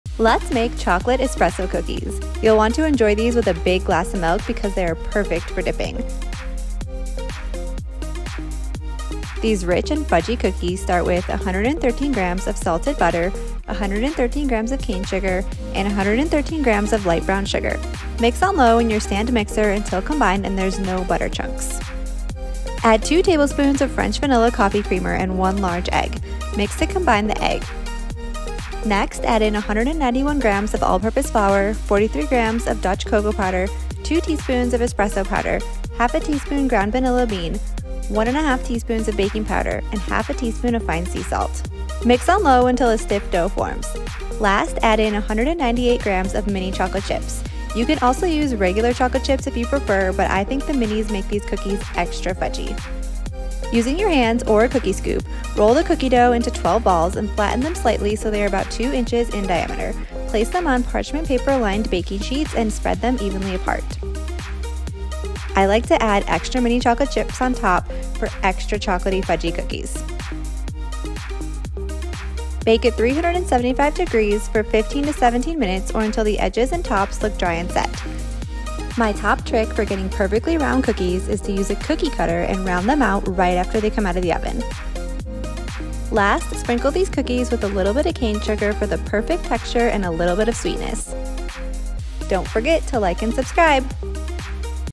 Chocolate Espresso Cookies — Rich, fudgy, and soft!
These soft and fudgy Chocolate Espresso Cookies are rich and chocolatey with a hint of espresso and lots of mini dark chocolate chips. You’ll want a big glass of milk with these!

🍪 RECIPE
113 grams salted butter, softened
113 grams cane sugar
113 grams light brown sugar
2 tablespoons vanilla coffee creamer
1 large egg
191 grams all purpose flour
43 grams Dutch cocoa powder, sifted
2 teaspoons espresso powder
1/2 teaspoon ground vanilla bean
1 1/2 teaspoons baking powder
1/2 teaspoon fine sea salt
198 grams mini chocolate chips